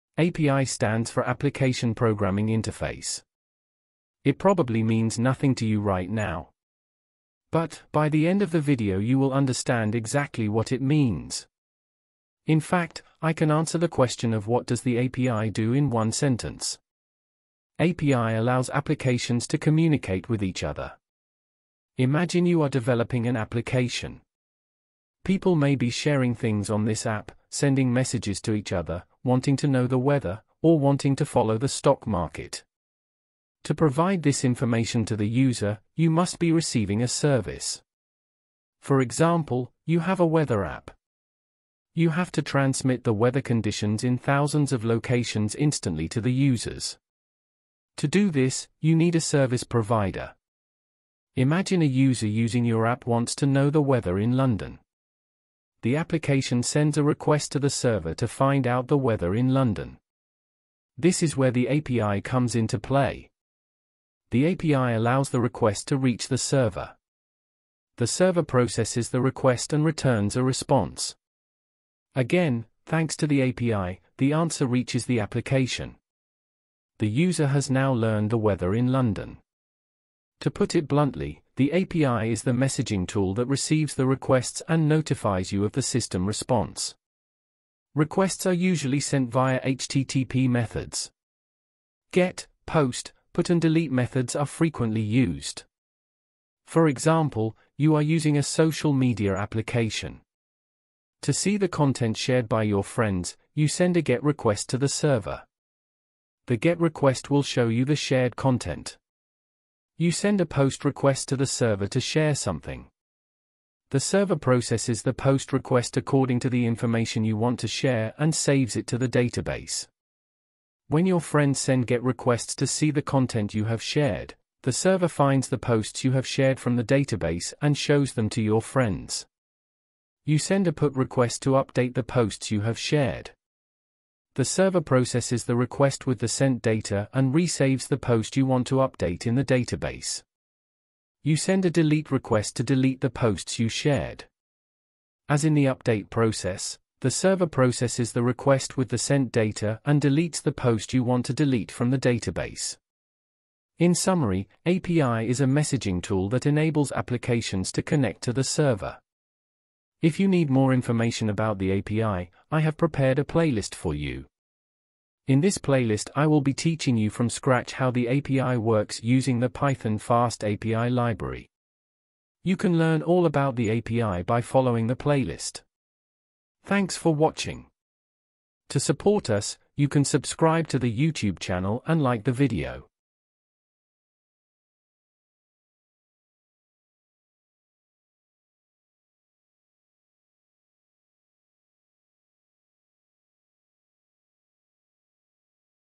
What is an API? - Python FASTAPI Tutorial 1
Everyone who deals with technology has asked themselves the question of what is an API. In this video, the question of what is an API is answered. If you need more information about the API, you can follow the playlist on how the Python FASTAPI library works.